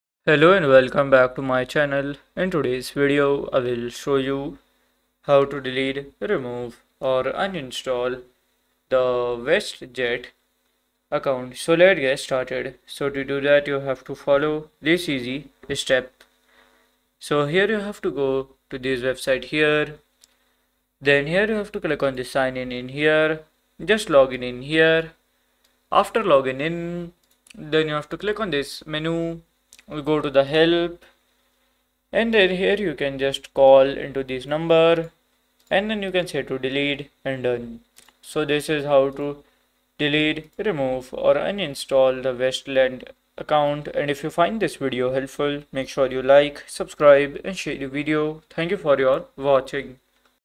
✅ How To Uninstall/Delete/Remove WestJet Account (Full Guide)
✅ How To Uninstall/Delete/Remove Vueling Airlines Account (Full Guide)

Learn how to uninstall, delete, or remove your Vueling Airlines account with our full guide. This video provides step-by-step instructions to help you permanently close your account.

Key Learning Points:
• Uninstalling Vueling Airlines Account
• Step-by-step deletion guide
• Tips for account management